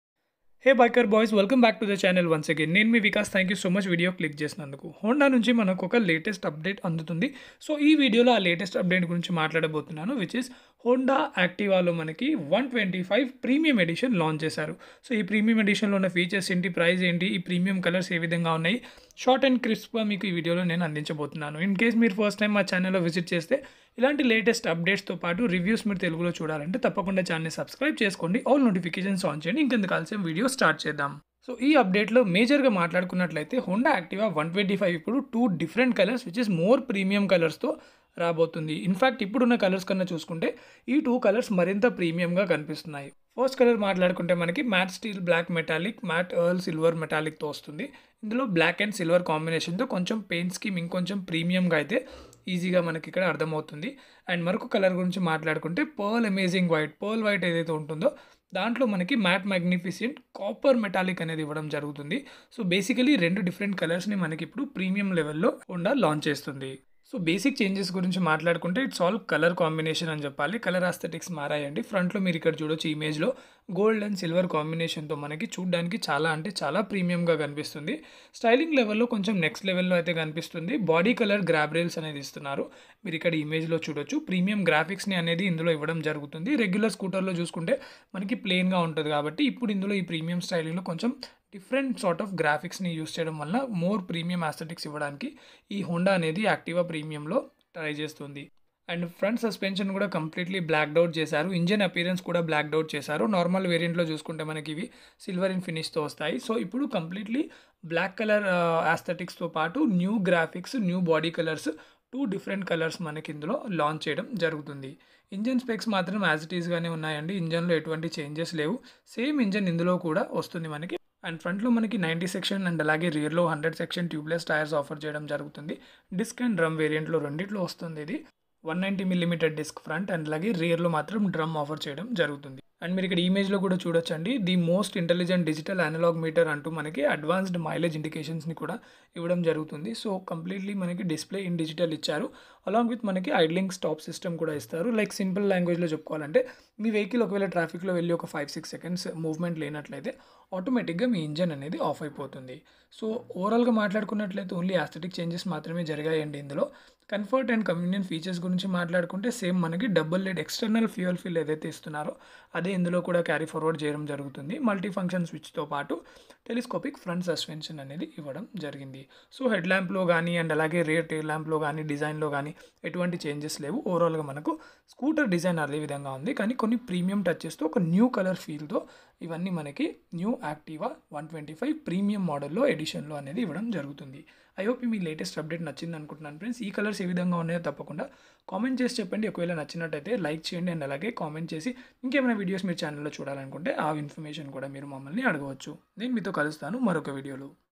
Activa 125 Premium Edition | Launched in India | Price Colors Specs in Telugu
Honda Activa 125 Premium Edition Launched in India : Price Colors Specs in Telugu